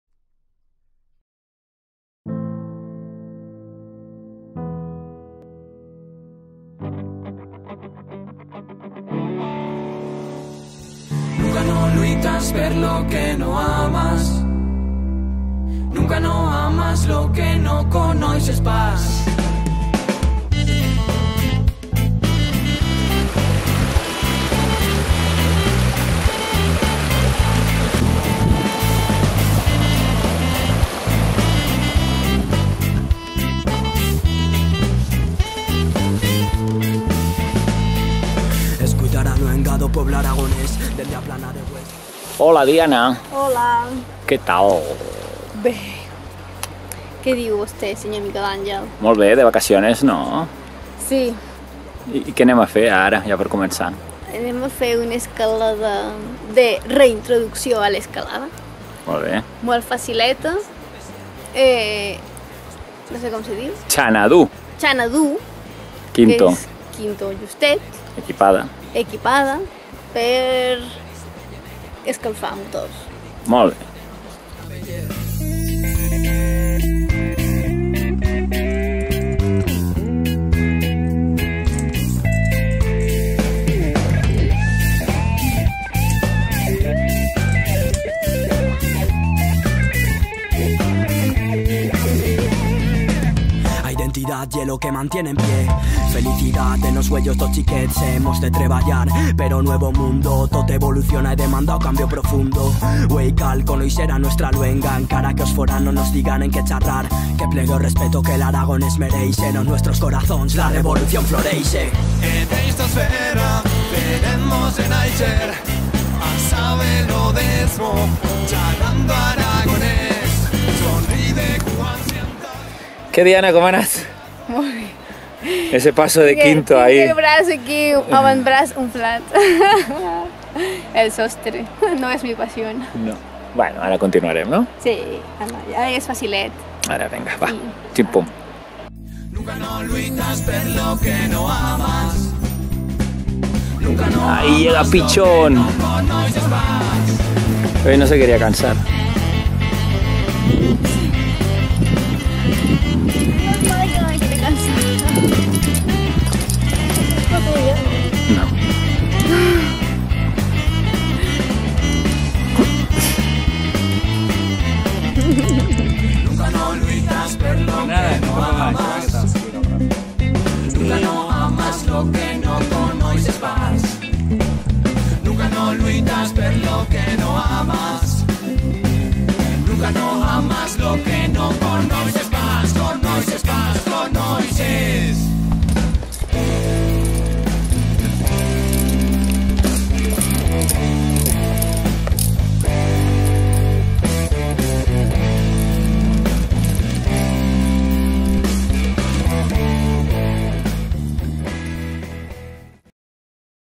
Via Xanadú a l'Ampriu (30.07.2022)
Aquesta via totalment equipada però no per això assequible, es troba a la vessant nord del Pic Cerler, a la zona coneguda com Palestras prop de l'Ampriu, a la Vall de Benasc.
Amb una dificultat que ronda el V/V+, ens tocarà apretar al principi del primer llarg i posteriorment en un sostret del segon llarg. El tercer llarg és una mica extrany.
Via ideal per una matinal o una tarda i que es pot combinar amb algunes properes.